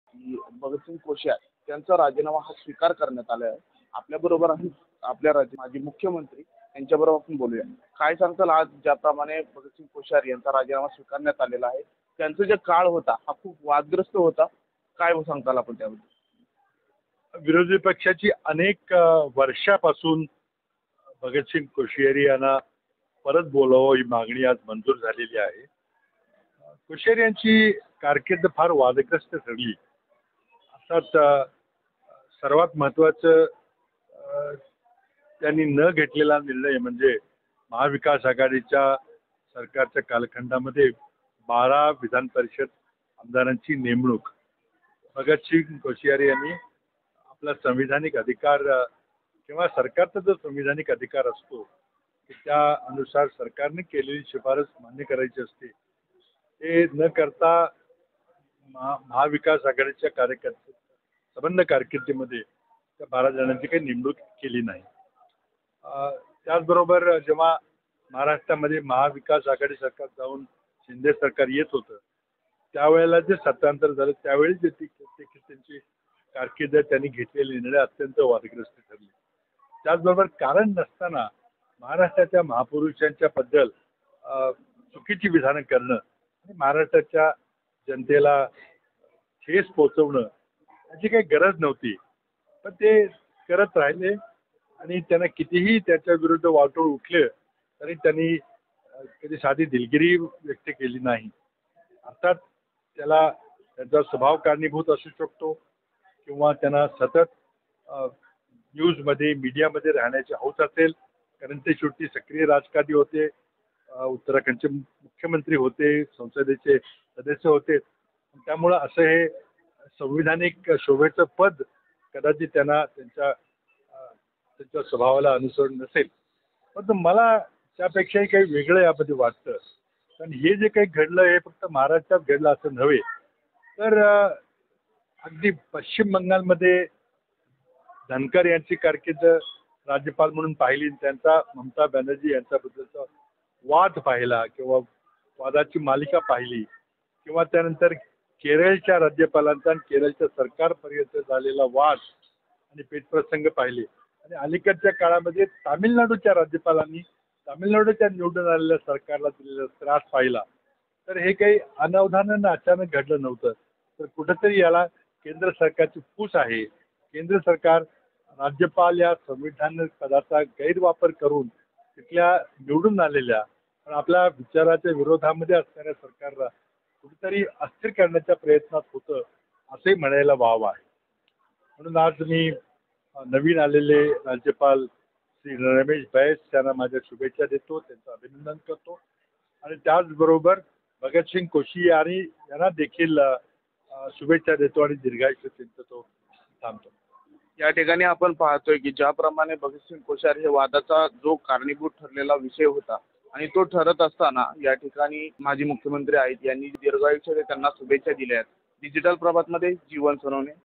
कोश्यरी यांनी राज्यपाल पदाची गरिमा नष्ट केल्याचा माजी मुख्यमंत्री पृथ्वीराज चव्हाण यांचा आरोप
कोश्यरी यांनी राज्यपाल पदाची गरिमा नष्ट केल्याचा माजी मुख्यमंत्री
पृथ्वीराज चव्हाण यांचा आरोप